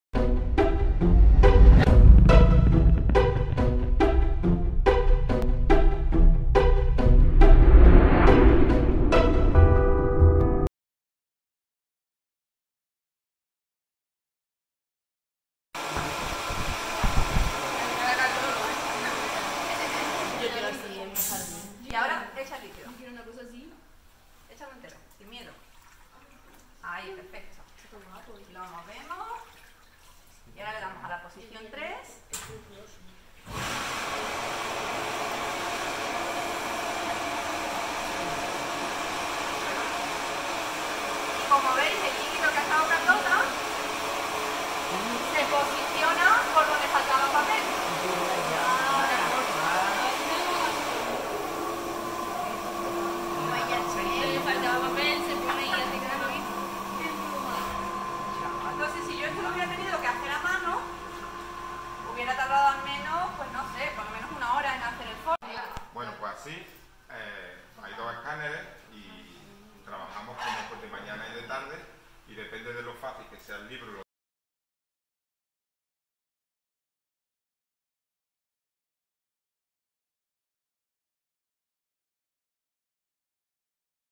VISITA A LA BIBLIOTECA DE SEVILLA
El alumnado de 1º de ESO en la materia de ASL con las profesoras Lito, Silvia e Hilda, han visitado la biblioteca de Sevilla realizando un interesante taller.